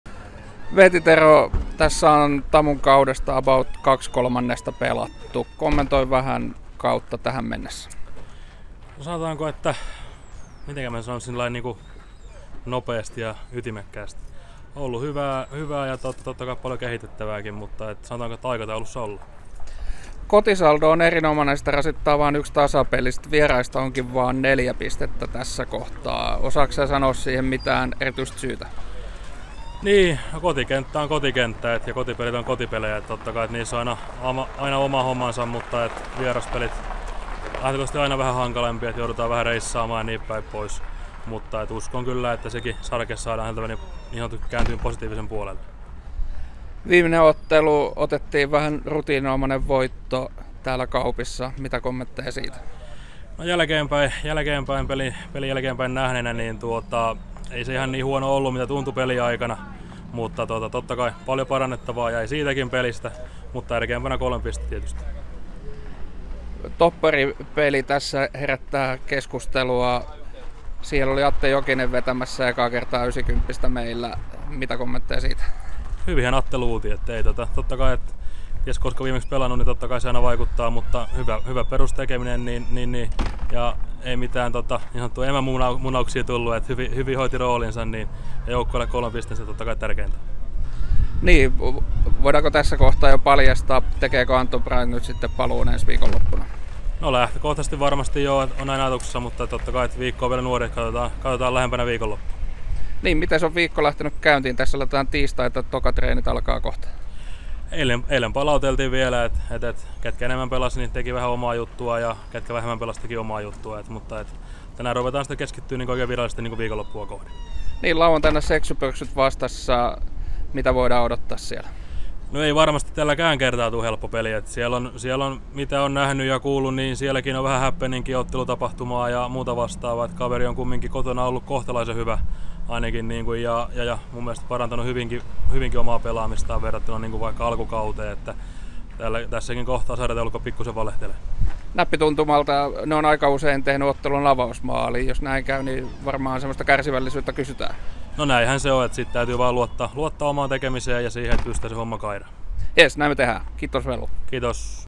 SexyPöxyt-ennakkohaastattelu: Weeti Tero
TamU-valmentaja Weeti Tero käy läpi koko tähänastista kautta, viime lauantain PIF-voittoa sekä tulevaa SexyPöxyt-kohtaamista.